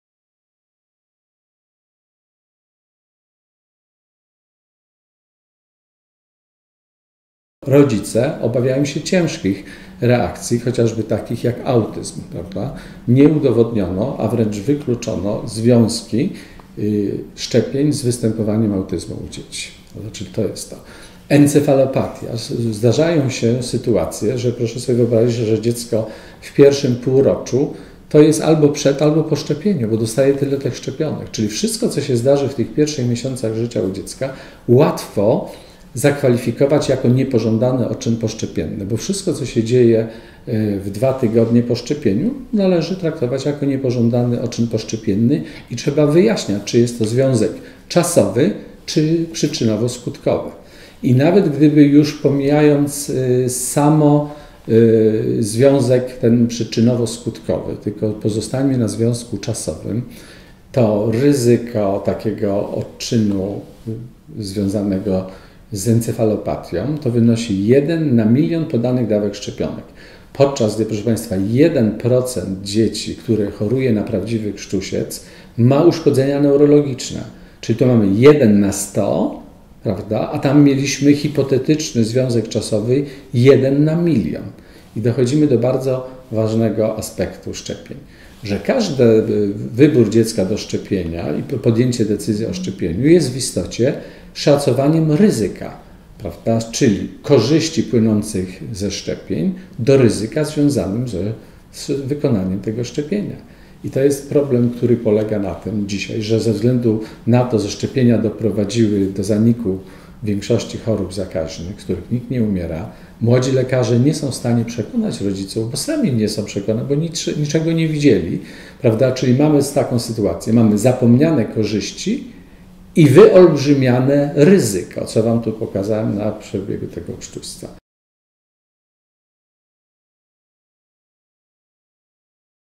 Pasja życia. Dbamy o zdrowie mam i dzieci - Czego obawiają się rodzice? - dr hab. Leszek Szenborn
Czego najczęściej obawiają się rodzice?
Na pytanie odpowiada pediatra dr hab. Leszek Szenborn profesor nadzwyczajny.